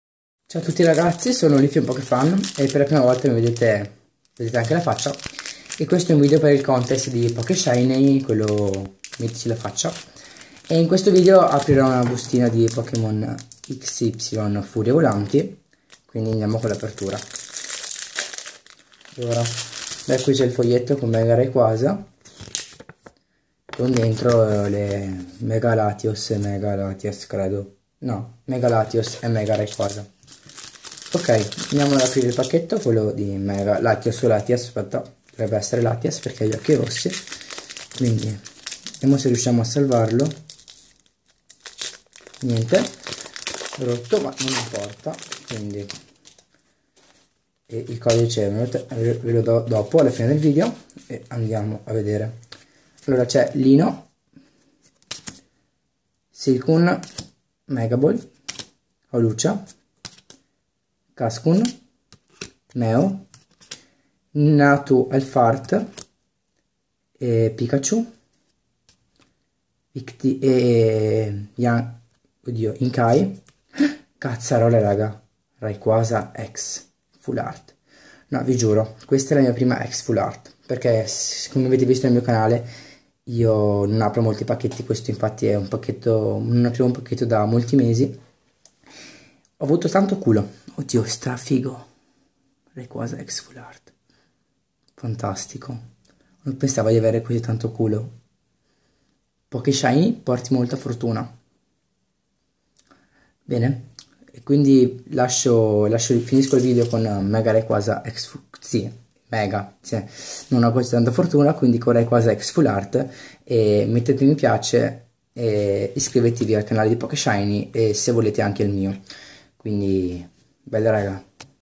VIDEO CONTEST DI LEAFEON POKEFAN: APERTURA BUSTINA POKEMON XY FURIE VOLANTI [EX FA]!
Ed ecco a voi ragazzi il primo coraggioso che "ci ha messo la faccia"!! ;-)) Avete tempo di dare i vostri likes fino al 26/06/2015!